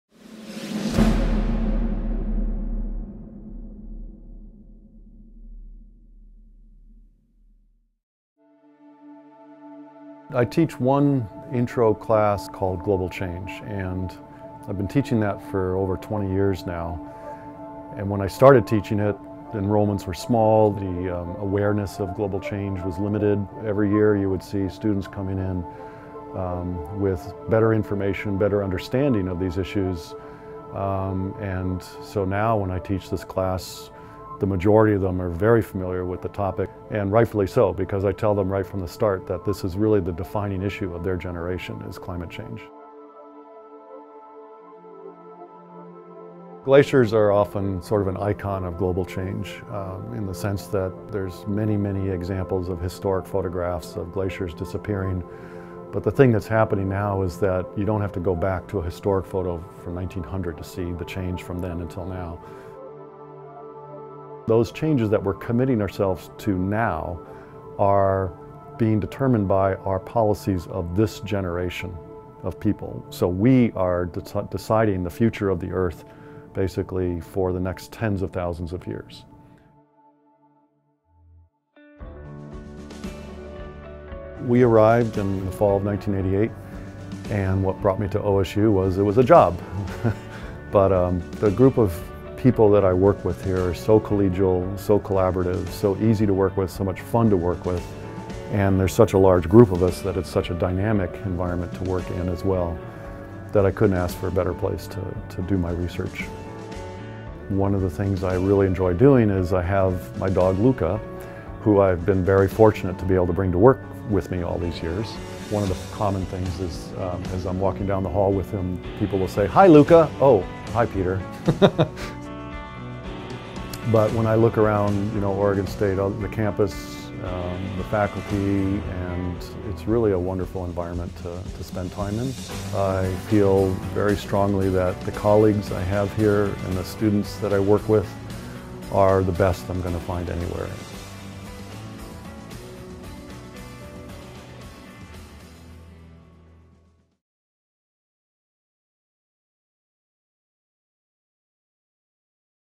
Distinguished Professor: Peter Clark
Clark, a professor in the OSU College of Earth, Ocean and Atmospheric Sciences, is an international leader in the study of past climate change to help understand what the future may bring. He has had numerous studies published in the most prestigious academic journals in the world, such as Science, Nature and Proceedings of the National Academy of Sciences. Clark also was a lead coordinating author for the Intergovernmental Panel on Climate Change.

With more than $4 million in research funding brought to OSU, Clark has studied glaciers and ice sheets, both those of today and from the distant past, to help determine what may be the long-term impacts of anthropogenic warming, rising greenhouse gases, and sea level rise. He’s also an award-winning teacher, recipient of 11 other major awards, has organized 20 symposia, and his professional work has generated literally thousands of citations.

Clark received his doctorate in geology from the University of Colorado and has been at OSU since 1988.